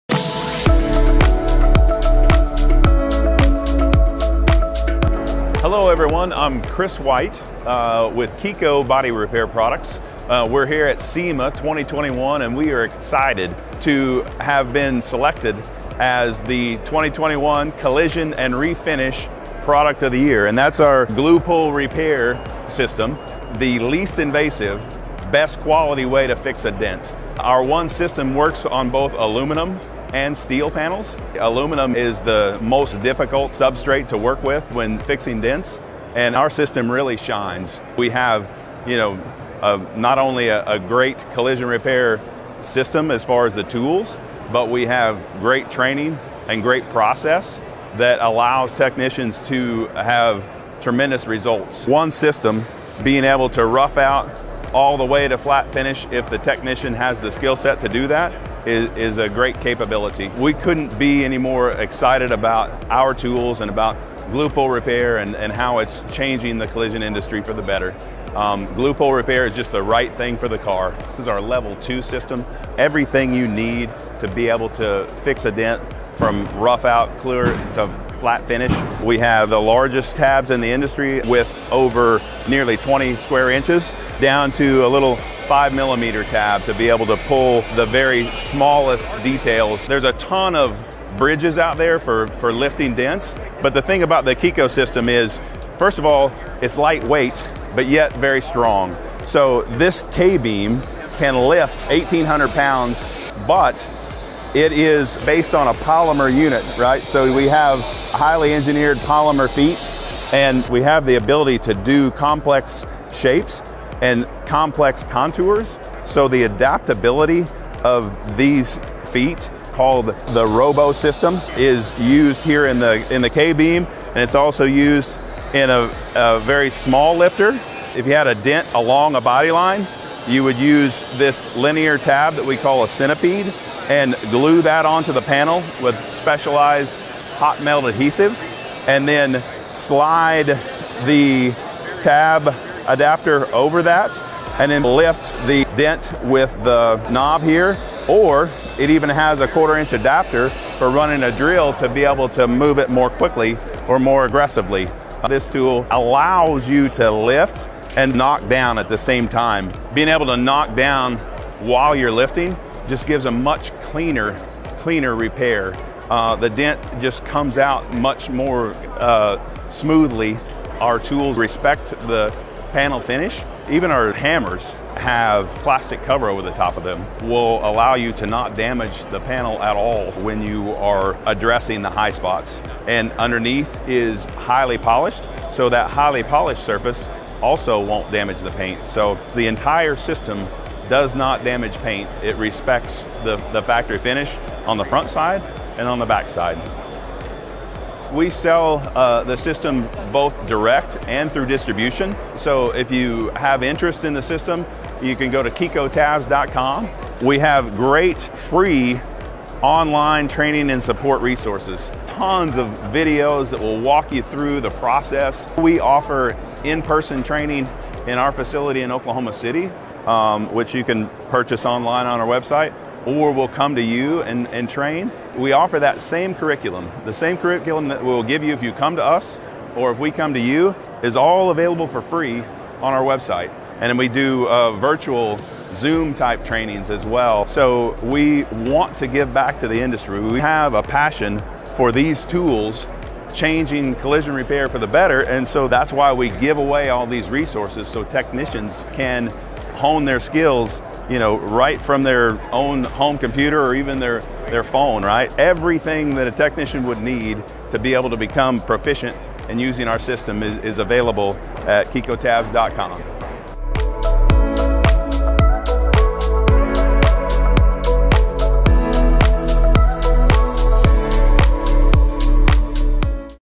KECO Highlights Glue Pull Repair System at SEMA Show
Chris White of KECO Body Repair Products discusses their Glue Pull Repair System, which SEMA named the 2021 Collision Repair & Refinish Product of the Year. White says the system is the least invasive and best quality way to fix a dent, and it works on both aluminum and steel.